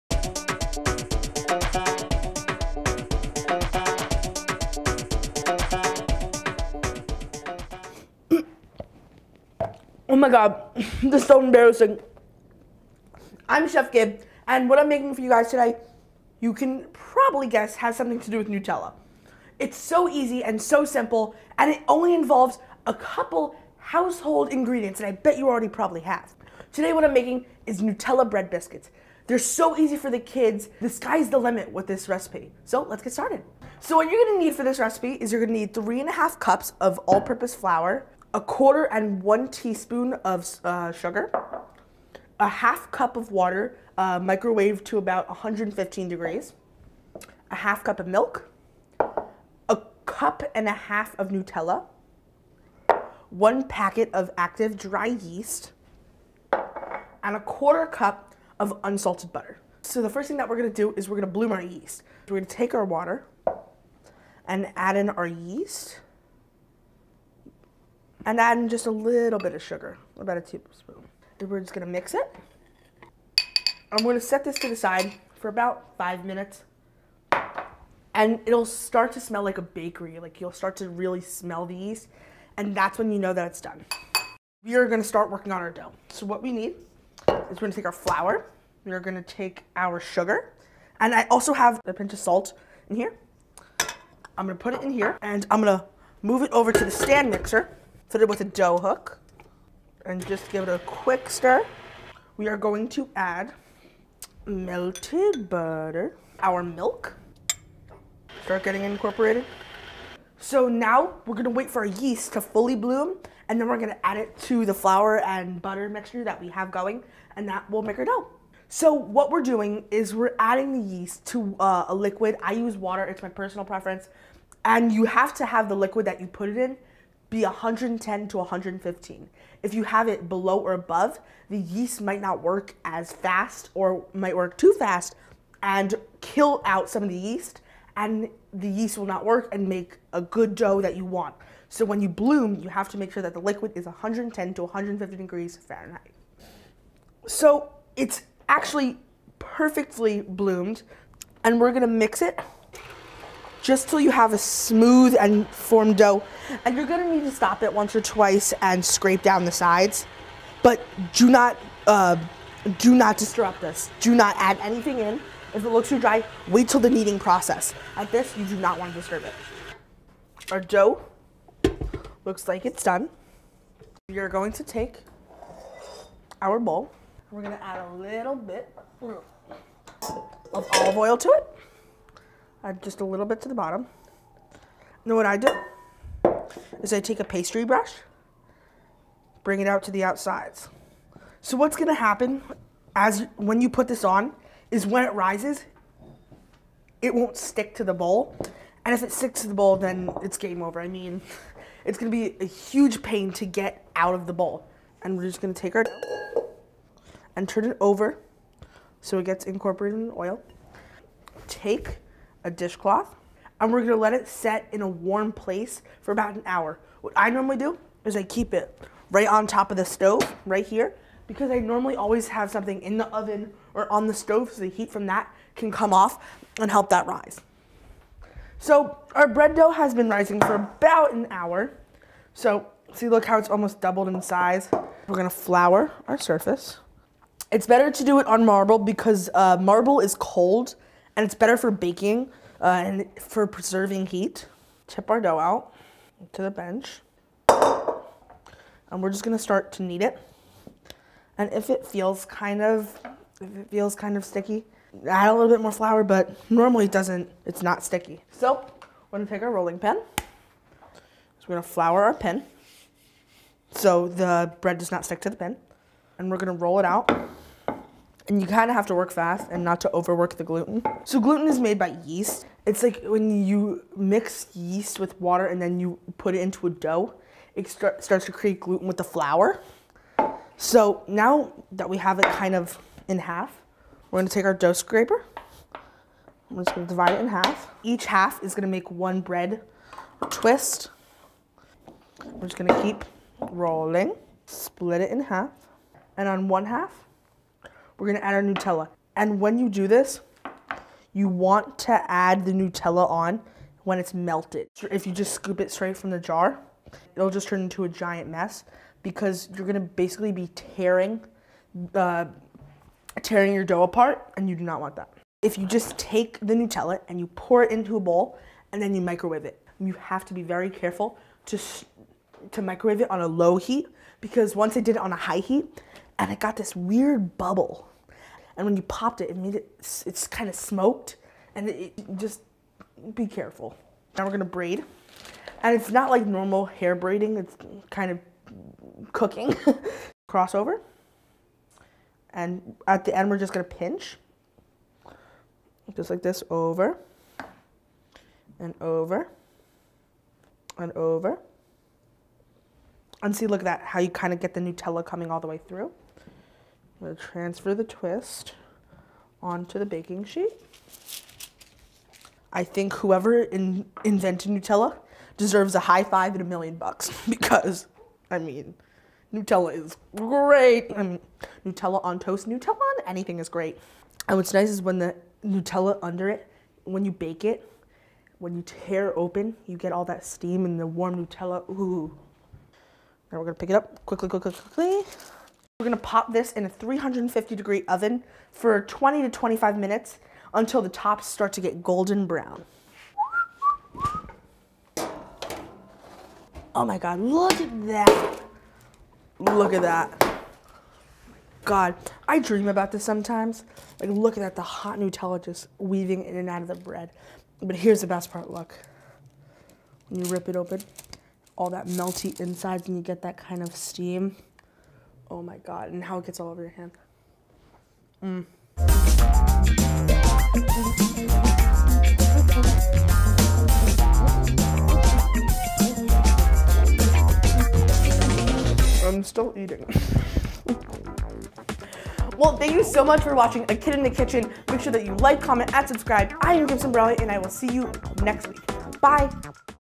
Kid in The Kitchen - Nutella Bread
Here's the second episode of my Kid In The Kitchen series where I bake up an awesome Nutella Bread. Best part is that Nutella is actually packed with Protein and good for you!